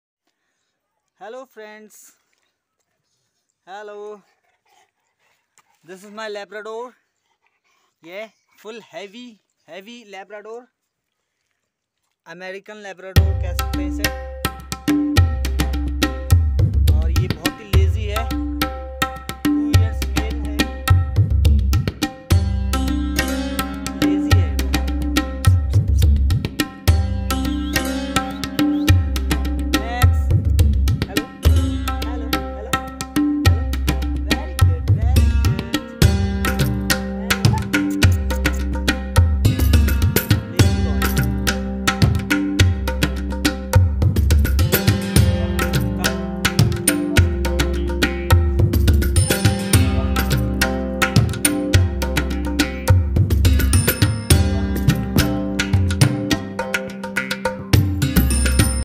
14 March 2022
Heavy bone American Labrador at Vk vlog and Dogs..Evening Play time with Max two years old Labrador.